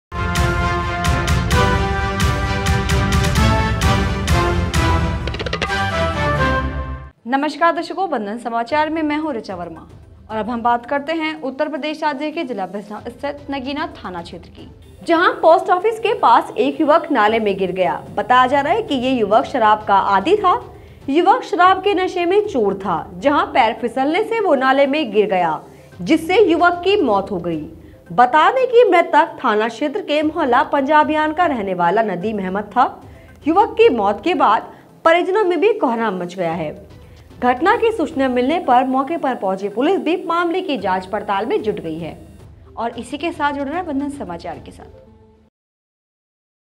शराब के नशे में चूर नाले में गिरा युवक, हुई मौत
नगीना के पोस्ट ऑफिस के पास एक युवक नाले में गिर गया, बताया जा रहा है ये युवक शराब का आदि था युवक शराब के नशे में चूर था, जहां पैर फिसलने से वो नाले में गिर गया, जिससे युवक की मौत हो गई|